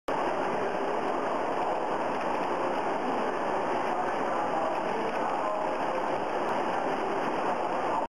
Pendulum Feeder
Short clip of a pendulum demand feeder (empty) on trout fry raceway